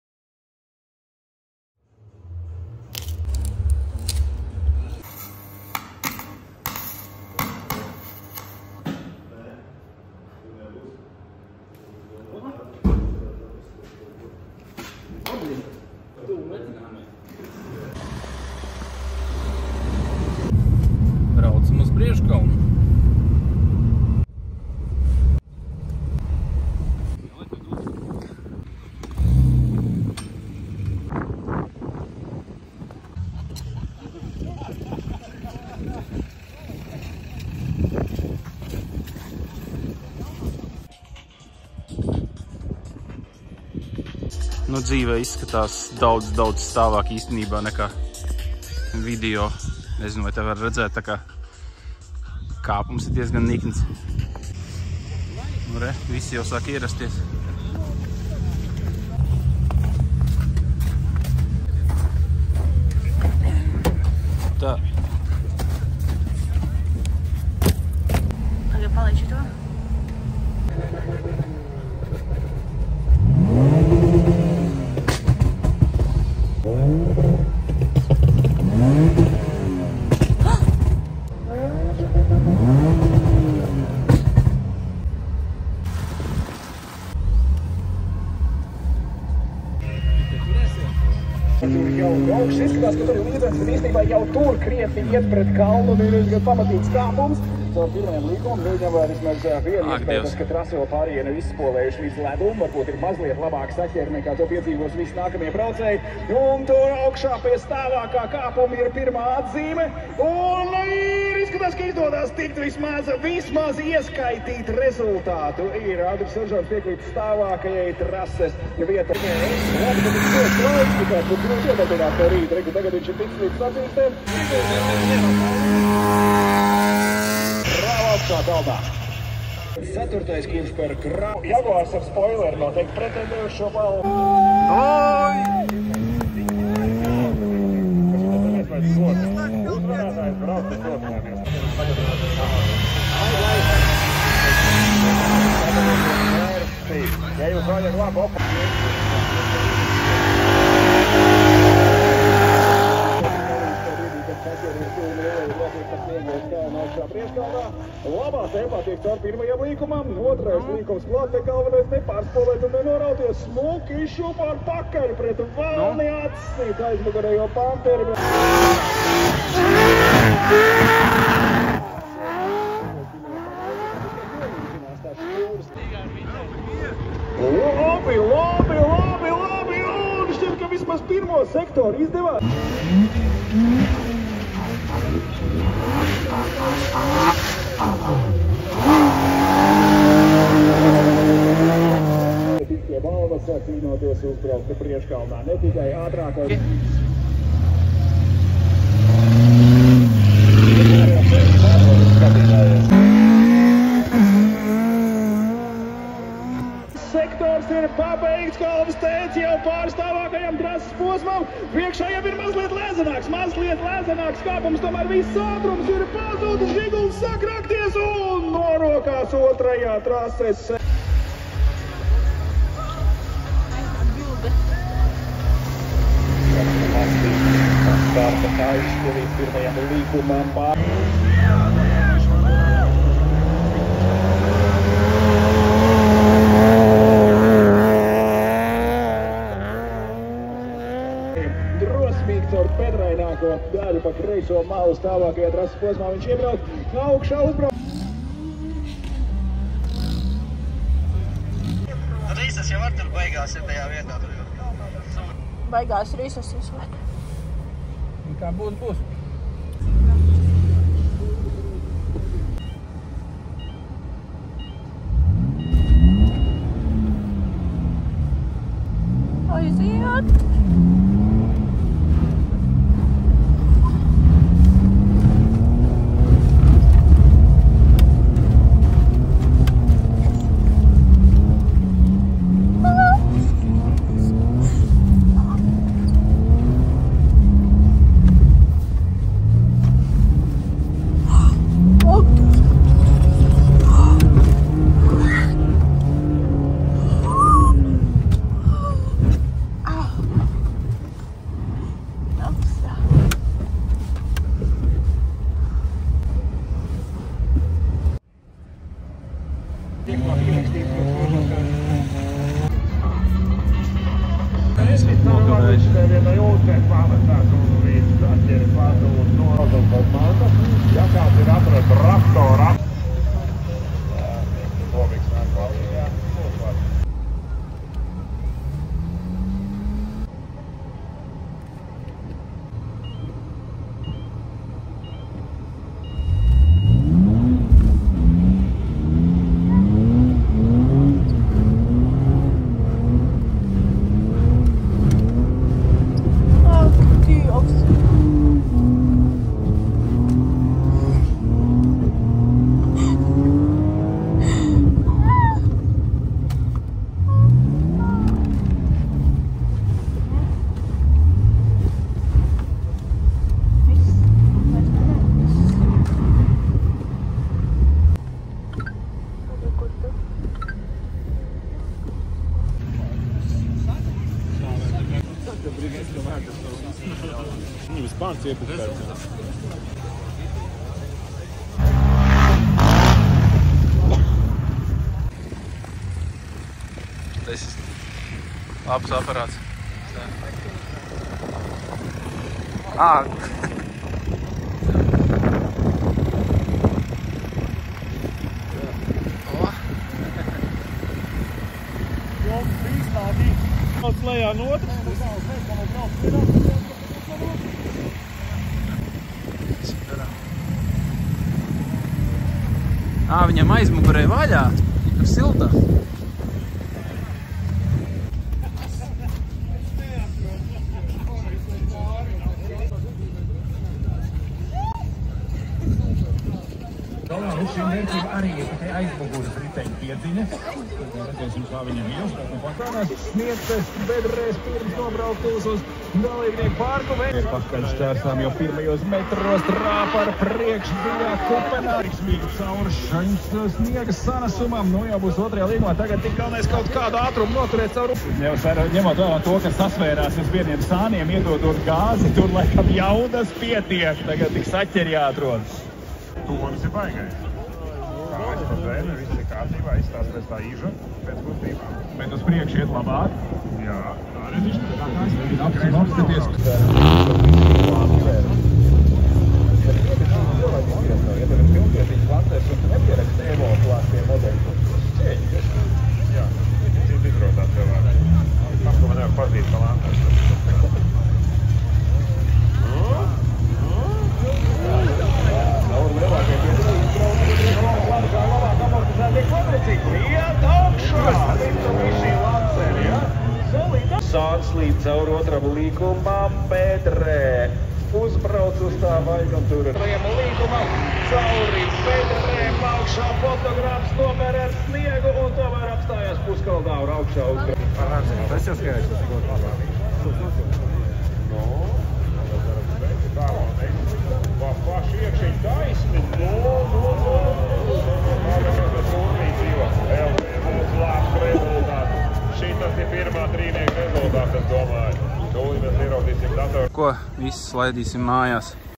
AUTOTRAKS 2024 BRAUCAM BRIEŽKALNĀ AR BMW
AUTOTRAKS 2024, BRAUCAM BRIEŽKALNĀ AR BMW 328 2WD klasē.

Bilde- stiprsfoto
Atbalstītāji- muzikālais Volvo, SIA Sistēmeksperts